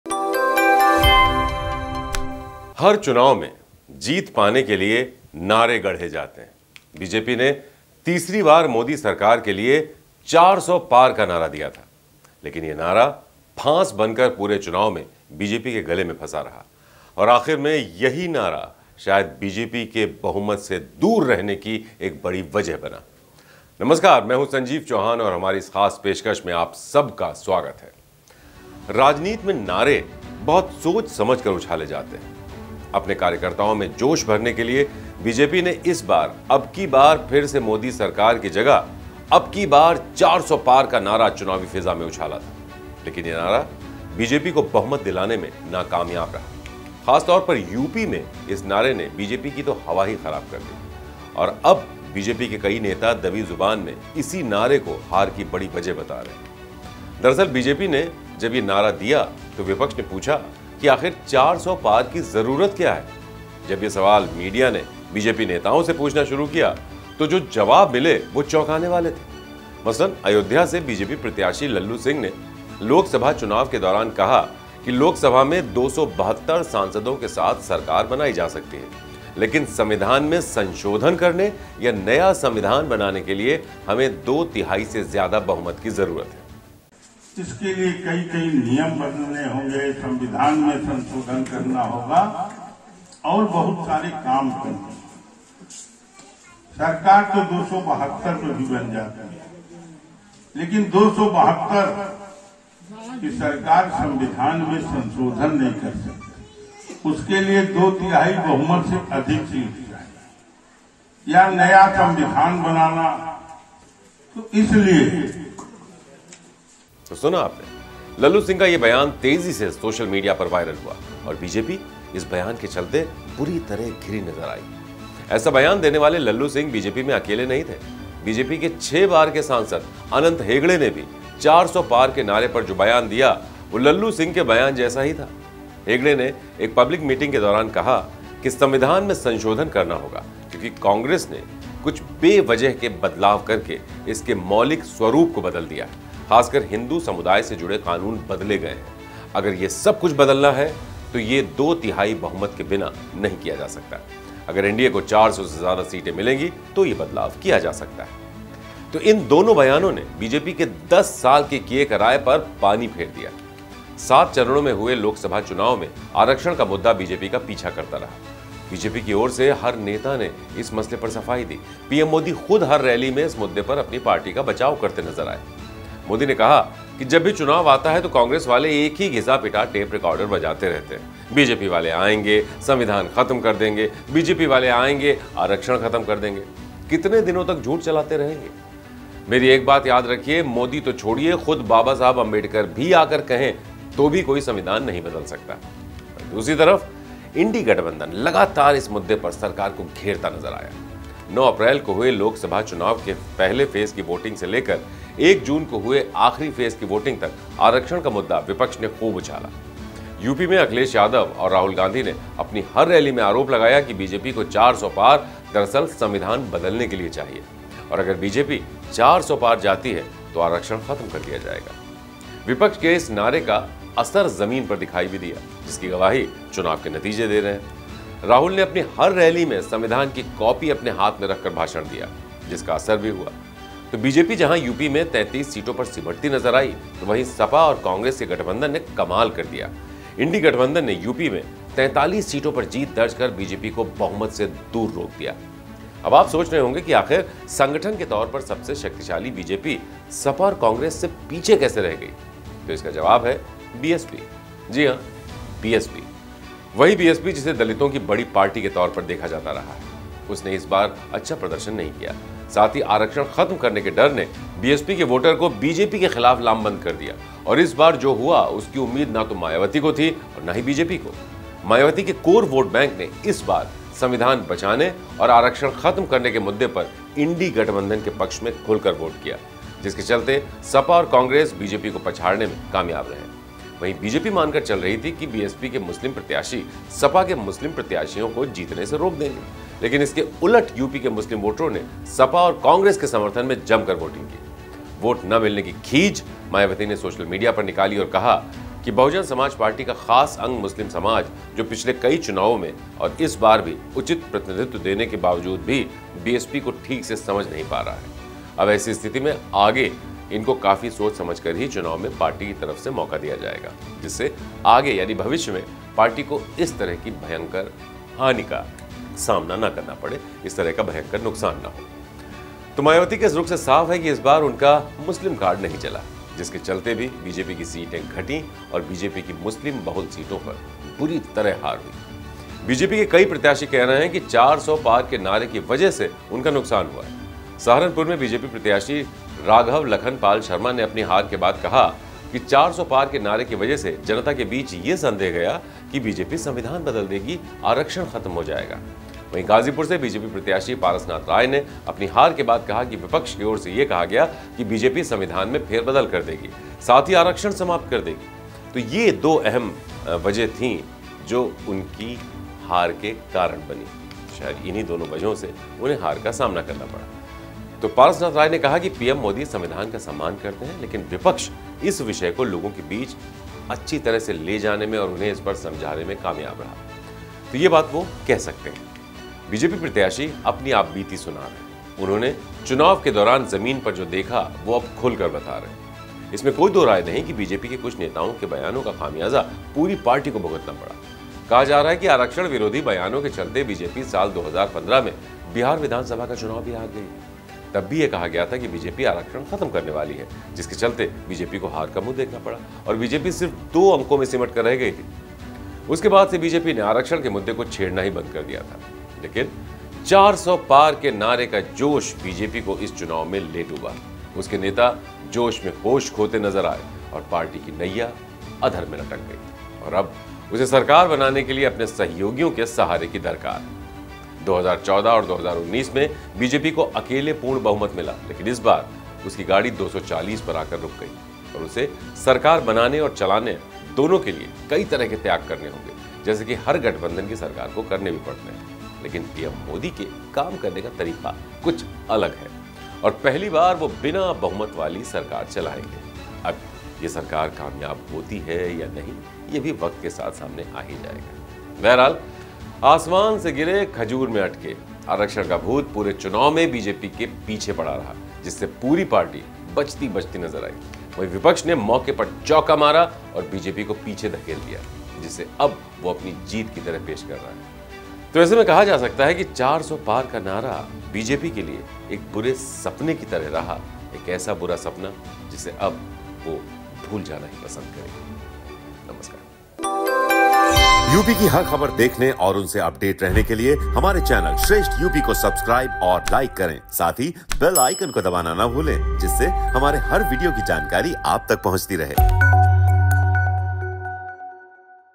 UP में BJP की हार की वजह 400 पार का नारा, Constitution और Reservation के मुद्दे पर 'INDIA' की जीत!
Loksabha Election Result 2024: हर चुनाव में जीत पाने के लिए नारे गढ़े जाते है..बीजेपी ने तीसरी बार मोदी सरकार के लिए 400 पार का नारा दिया था...लेकिन ये नारा फांस बनकर पूरे चुनाव में बीजेपी के गले में फंसा रहा..और आखिर में यही नारा बीजेपी के बहुमत से दूर रहने की बड़ी वजह बना है..